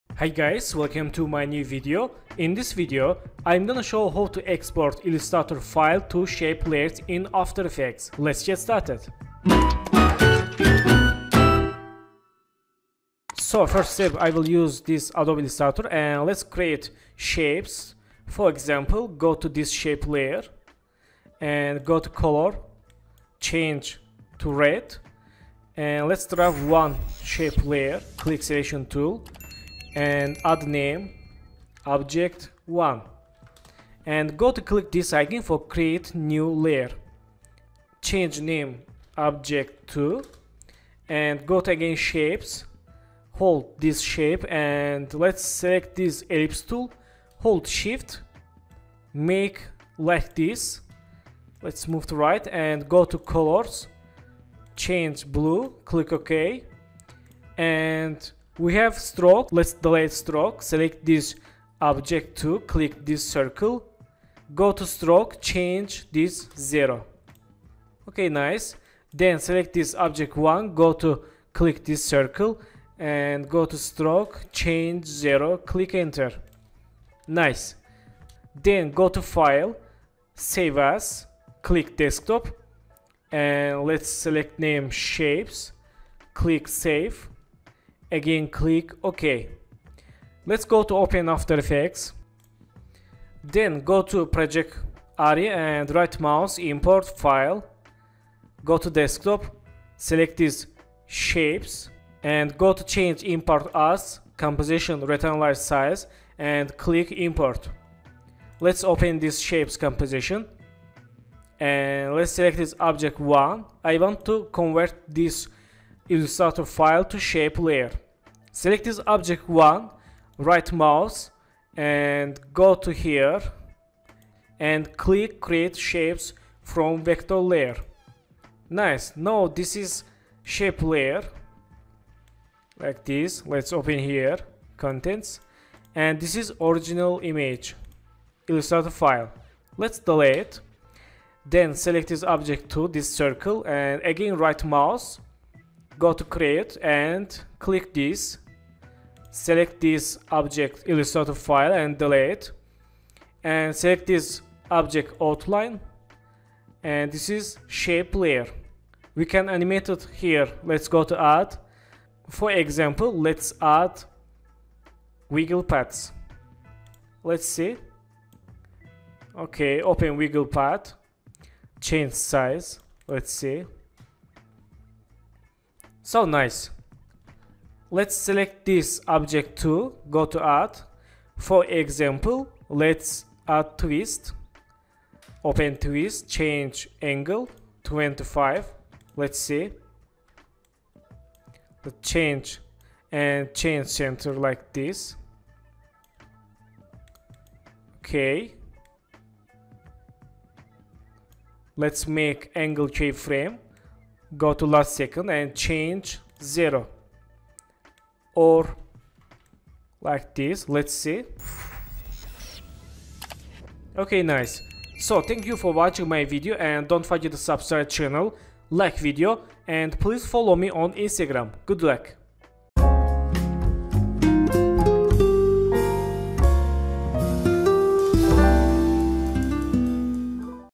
Illustrator Vectors Conver to After Effects Shape Layer
Hi guys, welcome to my new After Effects tutorial. In this tutorial i will show how to convert vector files to shape layers in After Effects. Let's get started!